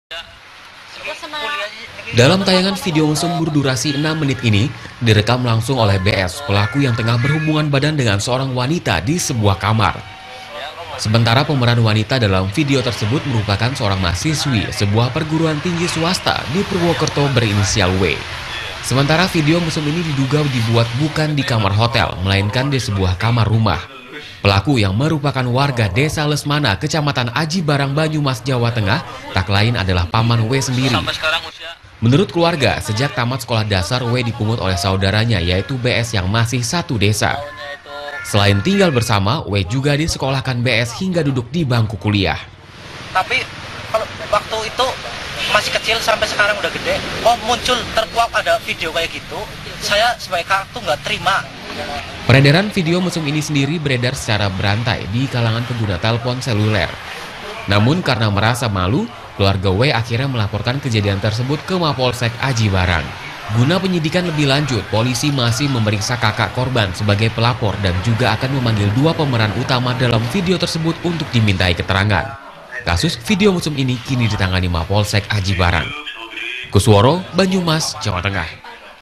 Video Mesum Paman dan Keponakan Bikin Heboh Banyumas Jawa Tengah
Warga Banyumas, Jawa Tengah dihebohkan dengan beredarnya video mesum yang beredar secara berantai melalui telepon seluler. Video berdurasi 6 menit itu diperankan oleh pasangan yang tak lain adalah paman dan keponakan.

Budayakan Like dan Coment

Silahkan di Subscrib Chanelnya untuk Video Lainnya